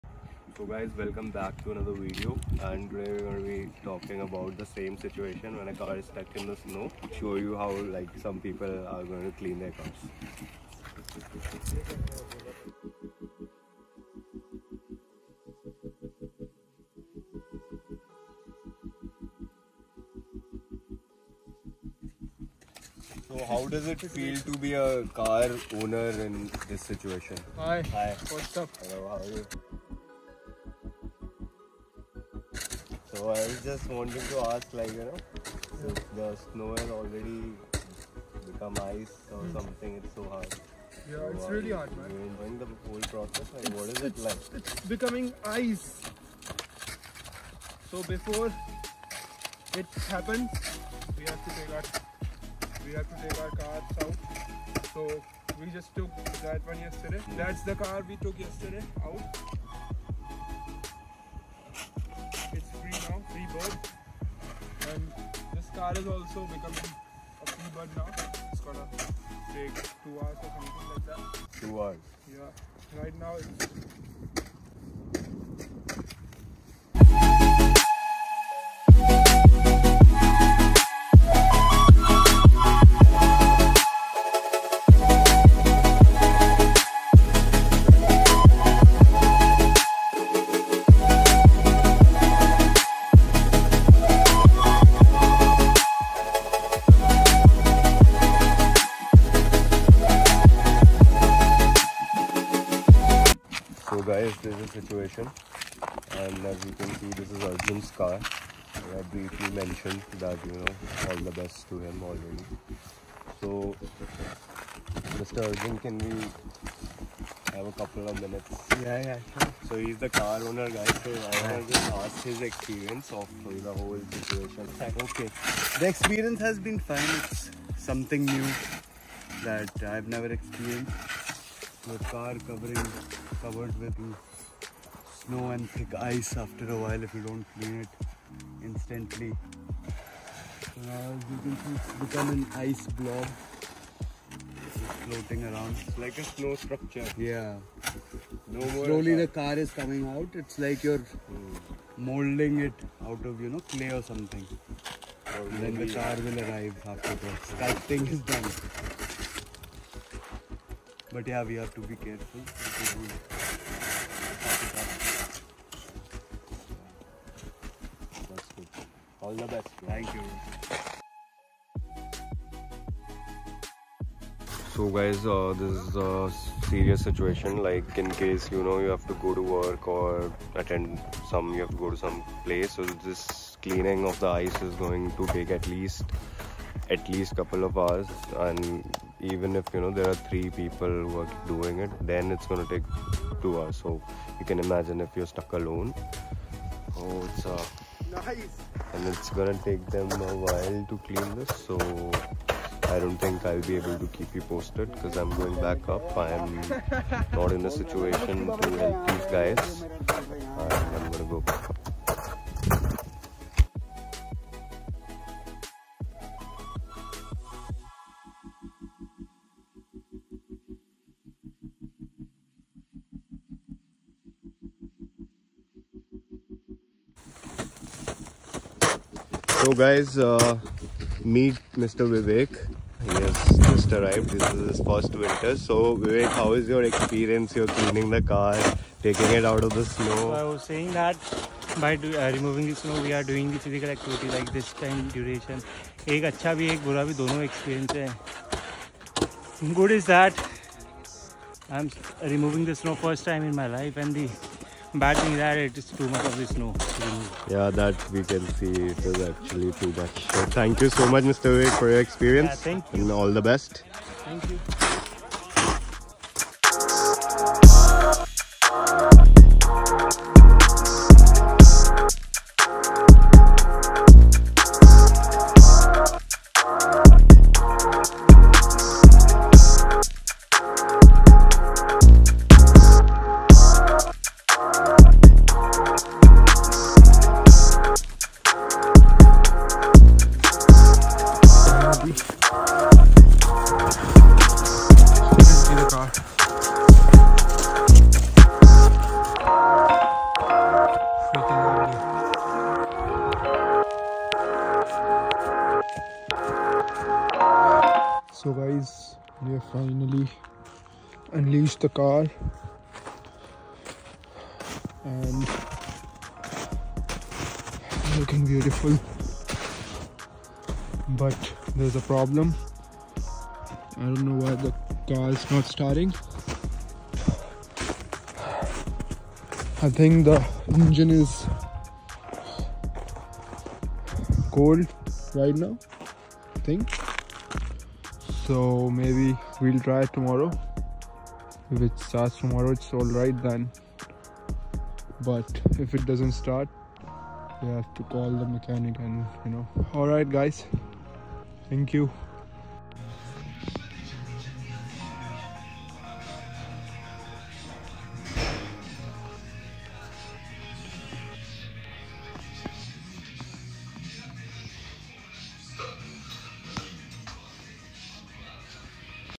The Snow car ❄️ ⛄️ Cape Breton University
Taking out Arjun’s car out of the snow as it was completely frozen under the ice from the past couple of days during the snow storm 🌨️
So we all got together and helped him out to get his car breathing again :)